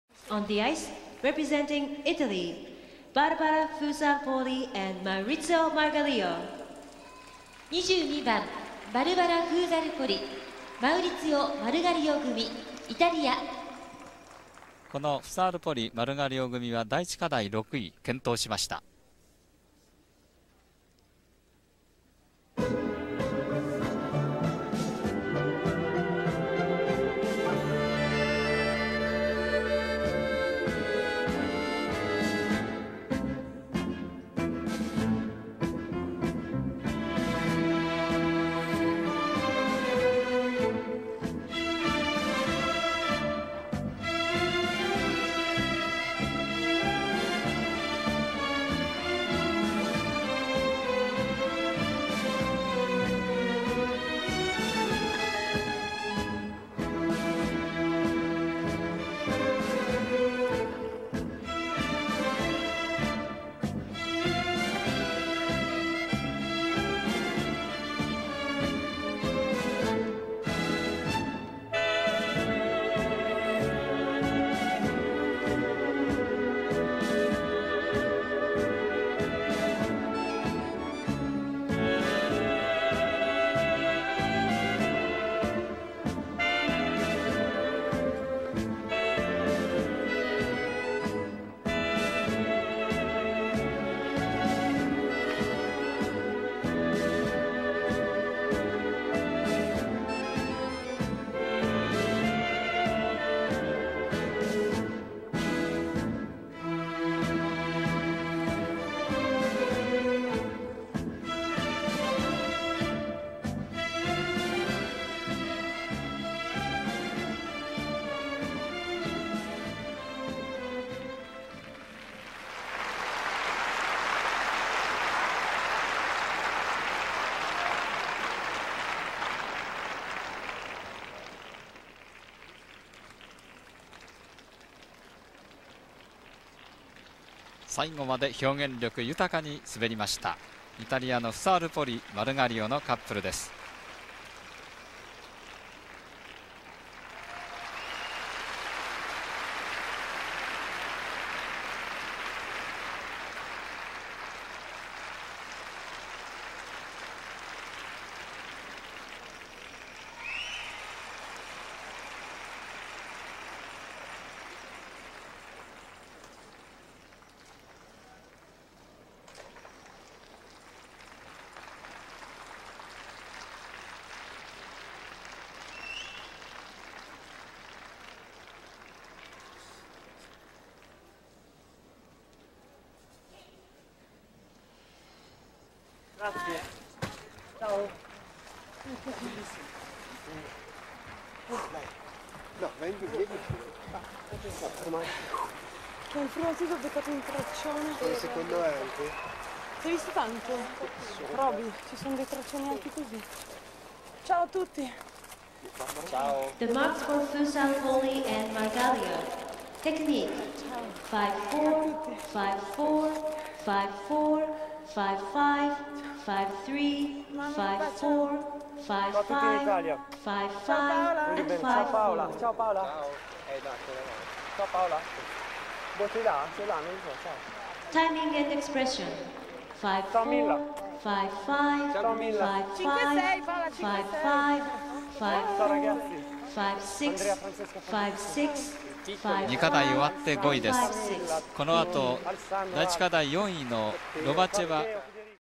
1998 Olympics. Barbara FUSAR-POLI - Maurizio MARGAGLIO. ITA. CD. Argentine Tango. 13.02.1998
Барбара Фузар-Поли и Маурицио Маргалио. Италия. Олимпийские игры 1998. Обязательный танец. Аргентинское танго